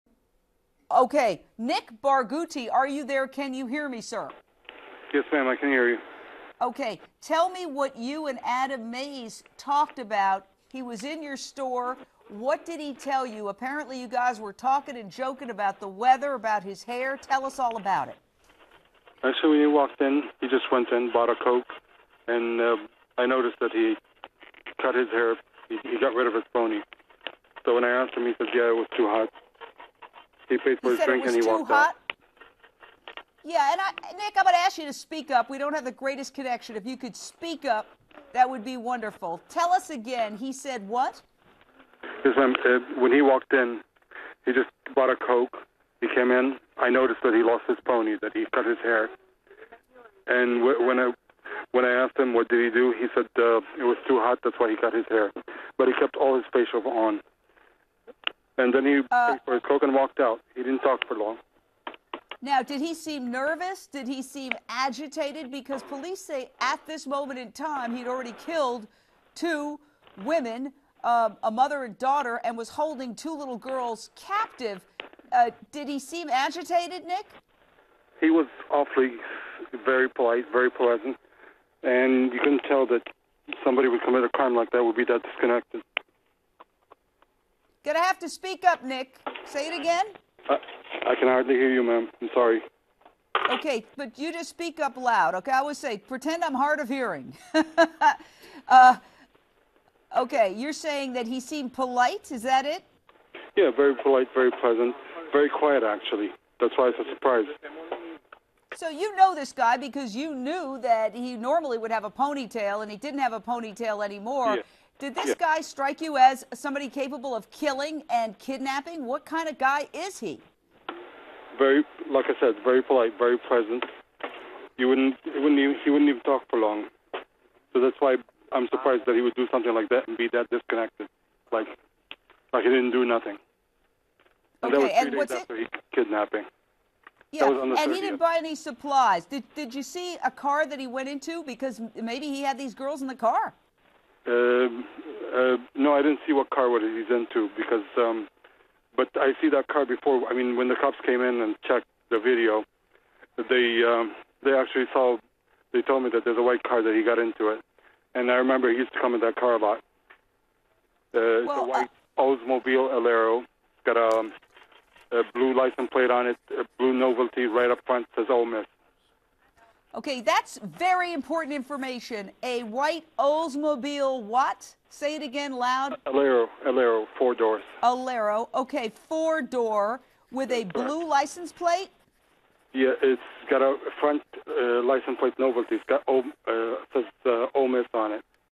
Update: Adam Mayes dead
*Update 9 p.m. ET -- Fugitive Adam Mayes is dead, and the two girls he was suspected of kidnapping are alive and safe, HLN learned Thursday night. The death was confirmed by FBI spokesman Joel Siskovic. There had been conflicting reports about Mayes since he was found after suffering an apparently self-inflicted gunshot wound Thursday night in Union County, Mississippi.A manhunt is underway for Adam Mayes who is accused of kidnapping and murder.  Mayes allegedly kidnapped Jo Ann Bain and her three daughters and then killed Jo Ann and the oldest girl.